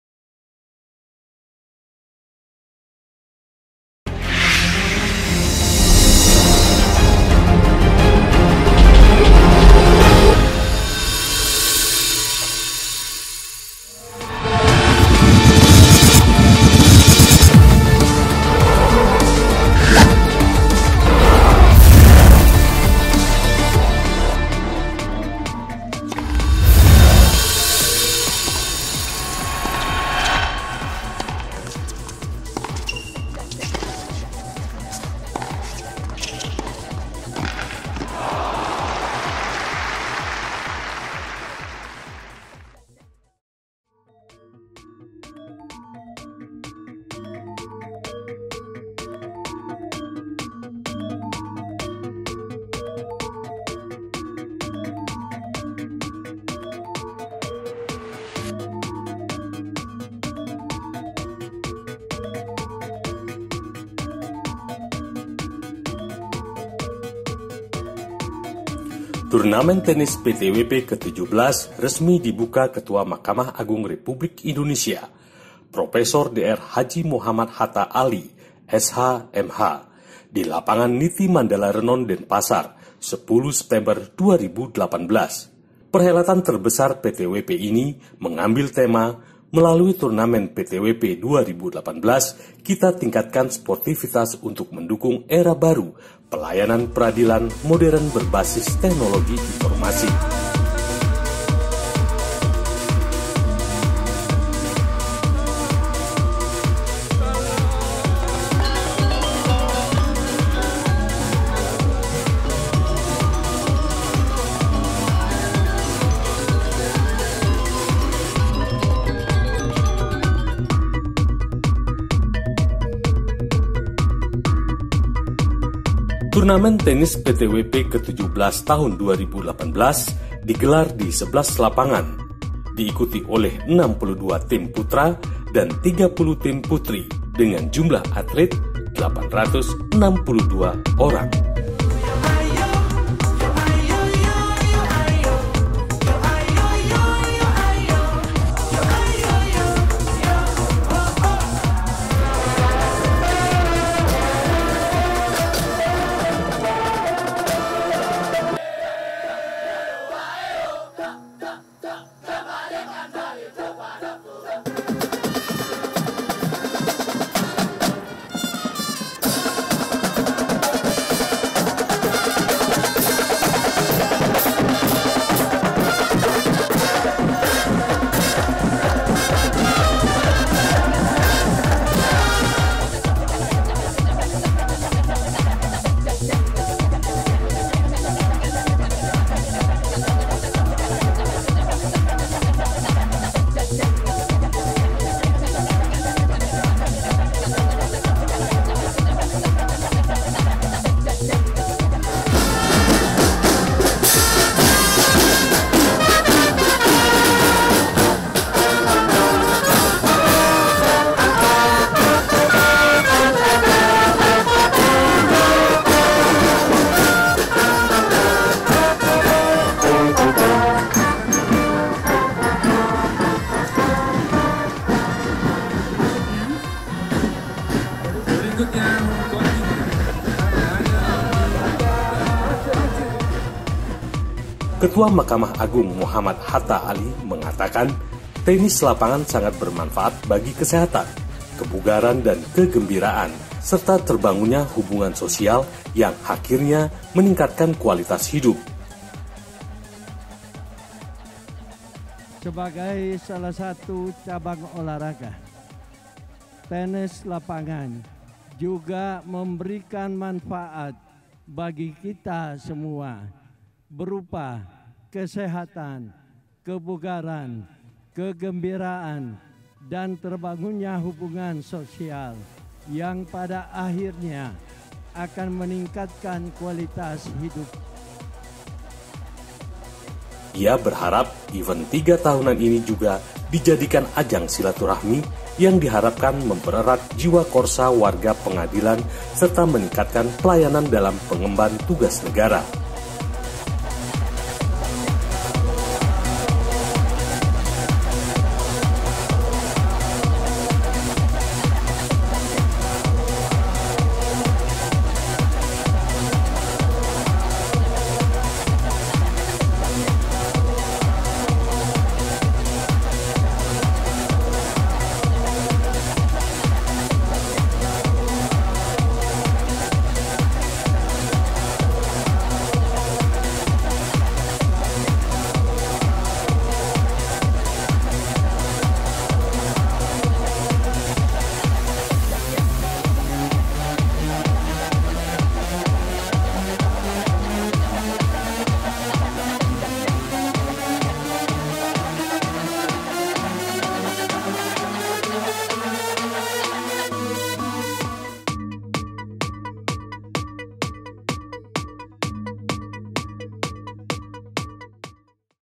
Opening Ceremony Turnamen Tenis PTWP XVII
Turnamen Tenis PTWP ke 17, resmi dibuka Ketua Mahkamah Agung Republik Indonesia Prof. Dr. H. M. Hatta Ali, S.H, M.H. di Lapangan Niti Mandala Renon Denpasar, 10 September 2018.
Perhelatan terbesar PTWP ini, mengambil tema "MELALUI TURNAMEN PTWP 2018, KITA TINGKATKAN SPORTIVITAS UNTUK MENDUKUNG ERA BARU PELAYANAN PERADILAN MODERN BERBASIS TEKNOLOGI INFORMASI".
Ketua Mahkamah Agung M. Hatta Ali mengatakan, Tenis lapangan sangat bermanfaat bagi kesehatan, kebugaran dan kegembiraan, serta terbangunnya hubungan sosial yang akhirnya meningkatkan kualitas hidup.
Ia berharap event 3 tahunan ini juga dijadikan ajang silaturahmi yang diharapkan mempererat jiwa korsa warga pengadilan serta meningkatkan pelayanan dalam mengemban tugas negara.
Turnamen Tenis PTWP ke 17 tahun 2018 digelar di 11 lapangan diikuti oleh 62 team putra dan 30 team putri dengan jumlah atlet 862 orang.
Organize by Jayaprana Pro